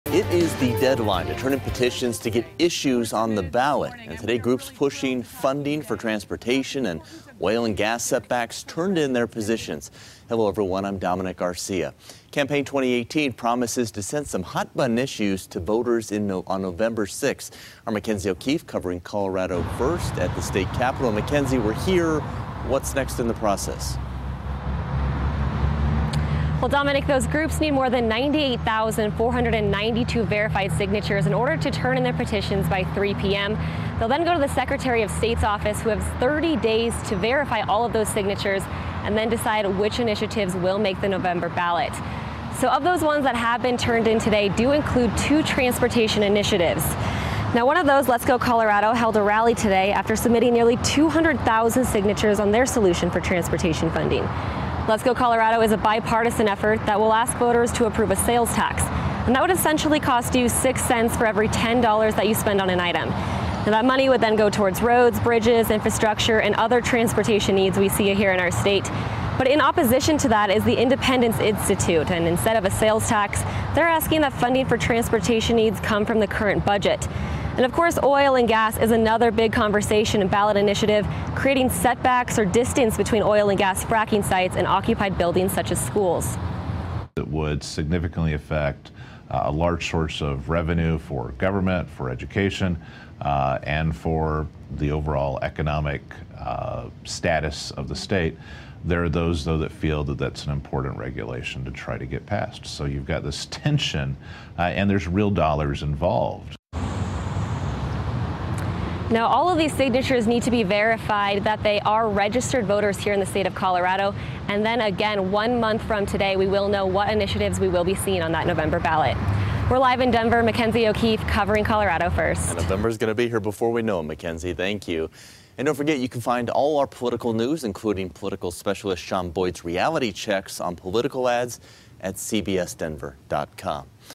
Aug. 6 Is The Deadline To Turn In Petitions For Ballot Measures
All signatures for petitions must be turned into the Secretary of State's Office on Monday.